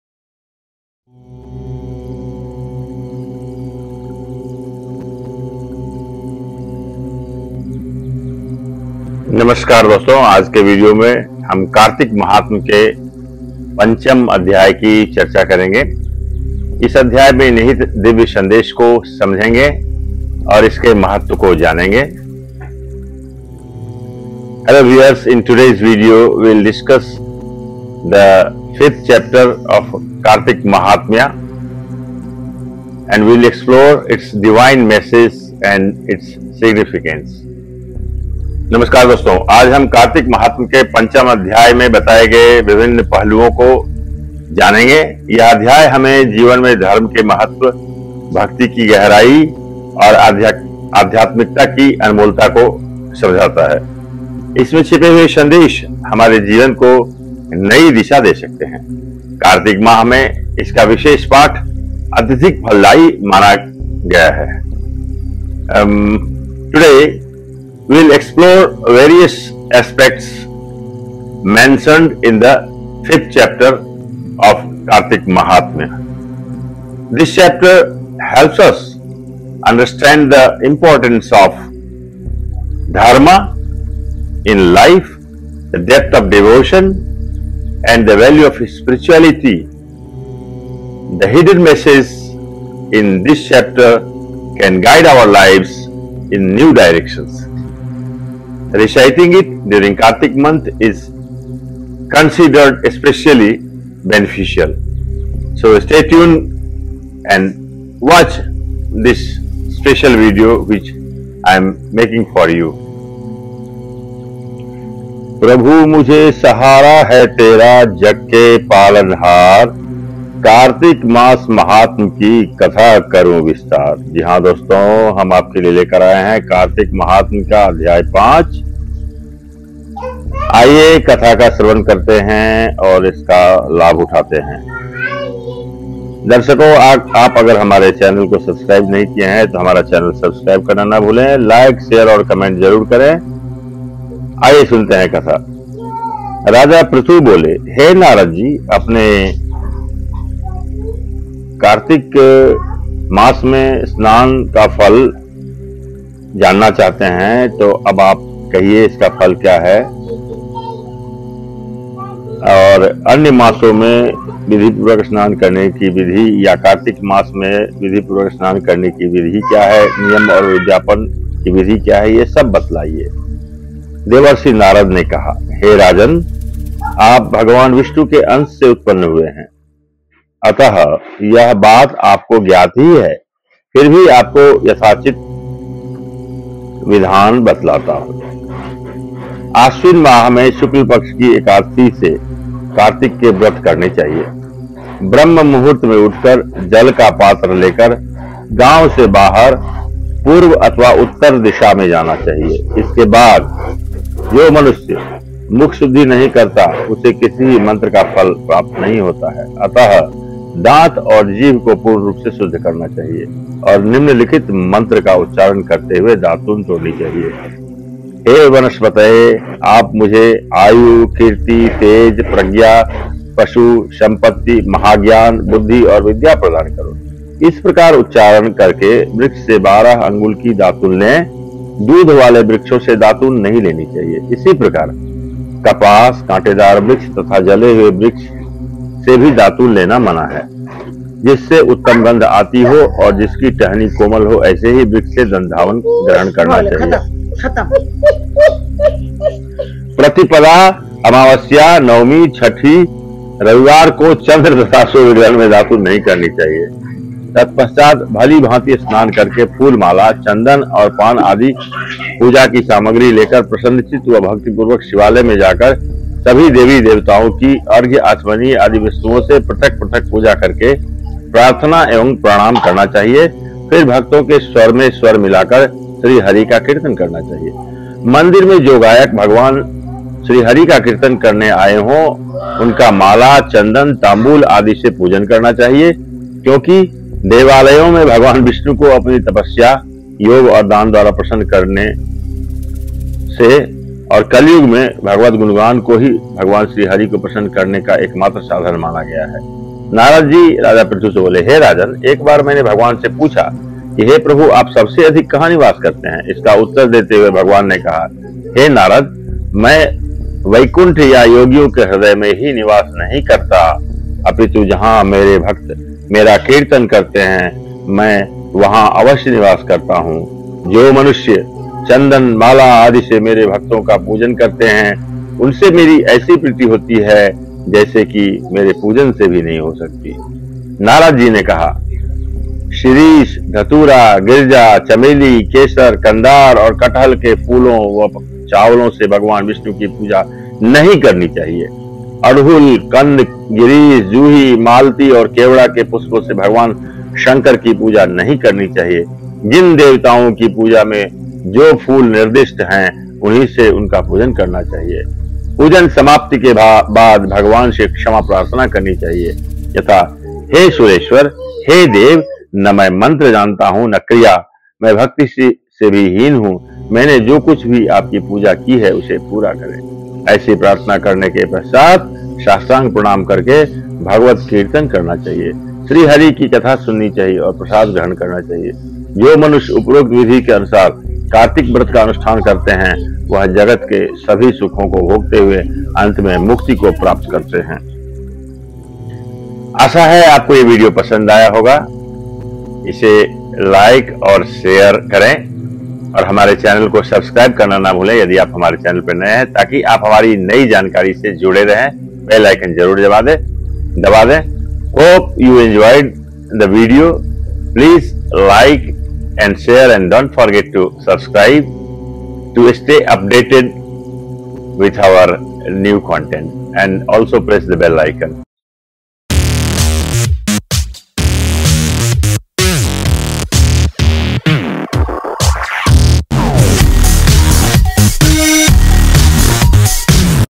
कार्तिक महात्म्य - Chapter 5 Insights
Hello Friends!
इस वीडियो में हम कार्तिक महात्म्य के पंचम अध्याय का सारांश और उसका महत्व समझेंगे। जानिए कैसे इस अध्याय में धर्म, भक्ति और अध्यात्म का संदेश दिया गया है। कार्तिक माह में इस अध्याय का पाठ विशेष लाभकारी माना गया है। इस अद्भुत कथा को देखें और पवित्र कार्तिक माह का पुण्य प्राप्त करें।

In this video, we delve into the essence of the Fifth Chapter of Kartik Mahatmya. Discover the deep messages of dharma, devotion, and spirituality. Reciting this chapter during the holy month of Kartik is considered highly auspicious. Watch this enlightening tale and gain blessings during Kartik month.